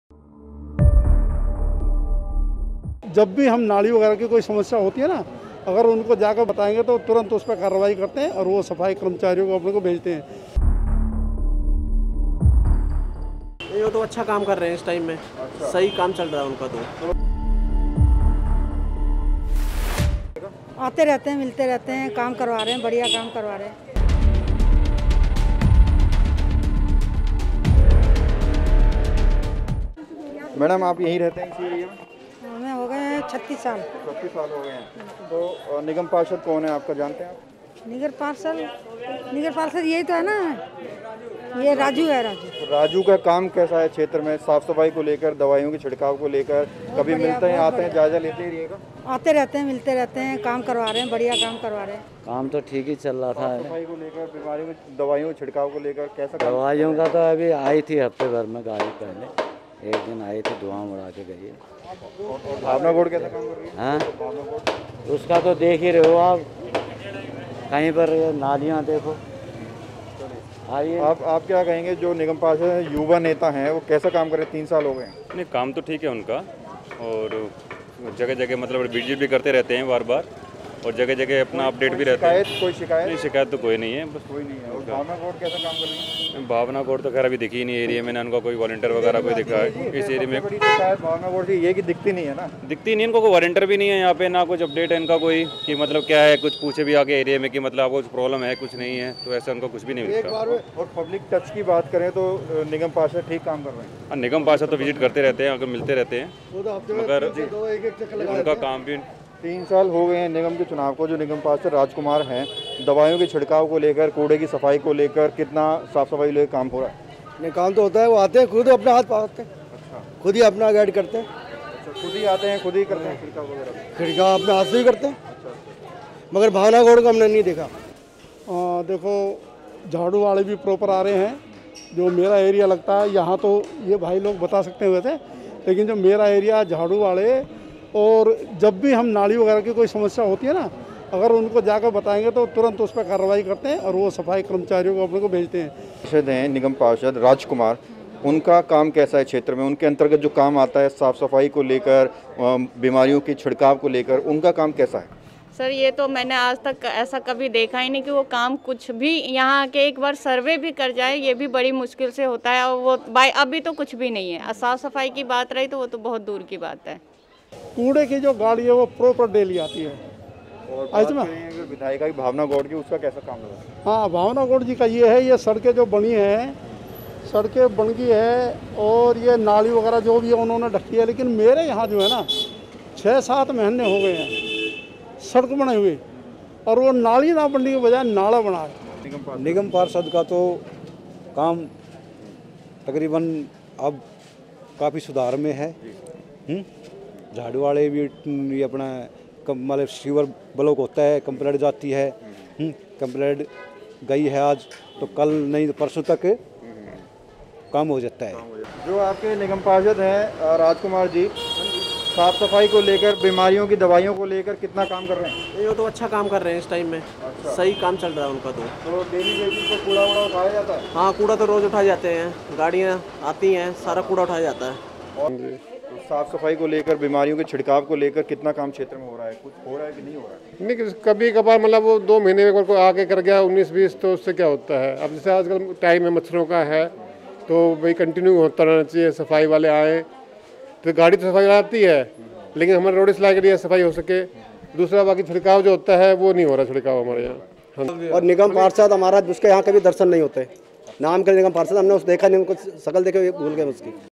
Palam: Ward 52s के निगम पार्षद Rajkumar के कार्यों के बारे में क्या कहती है जनता...
इंडिया टाइम 24 न्यूज़ ने पालम विधानसभा में आने वाले वार्ड 52 का सर्वे किया।  यहाँ लोगों ने अपने निगम पार्षद राजकुमार और विधायक भावना गॉड के कार्यों के बारे में निष्पक्ष राय दी।